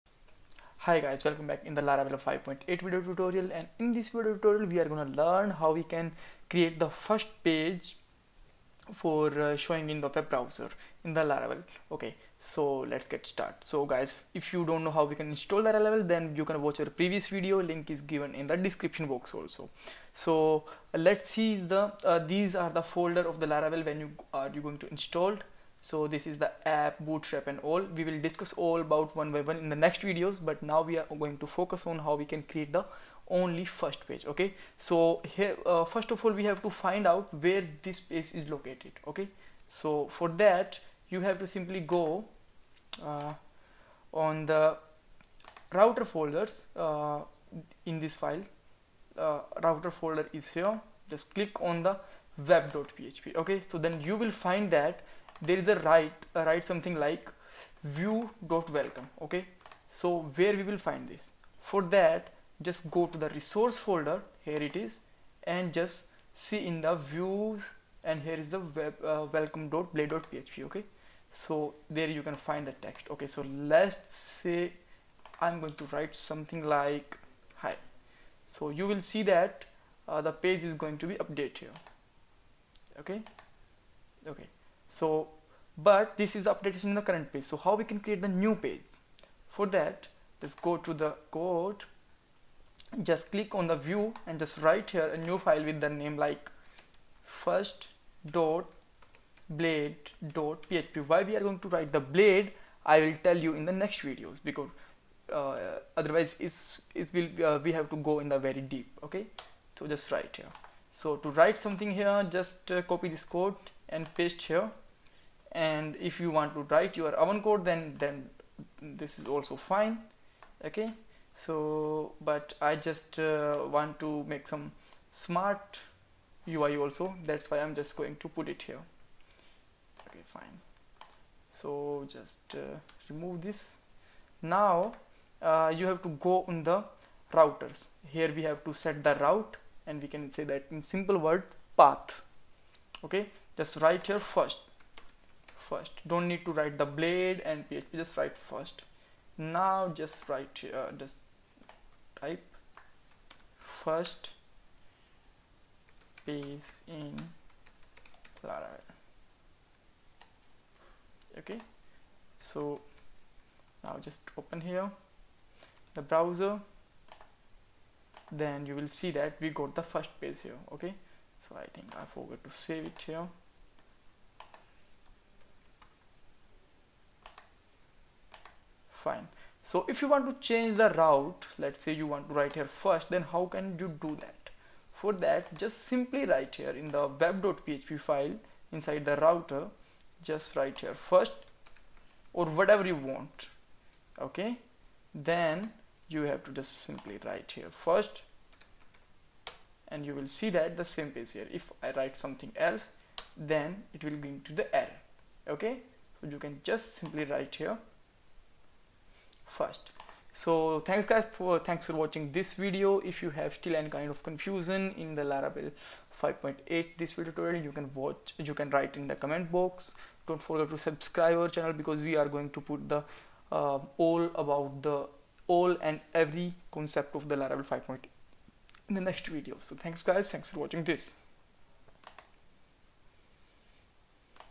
Laravel 5.8 tutorial #2- create first page
here in laravel 5.8 tutorial we will  learn how to createfirst laravel page with simple example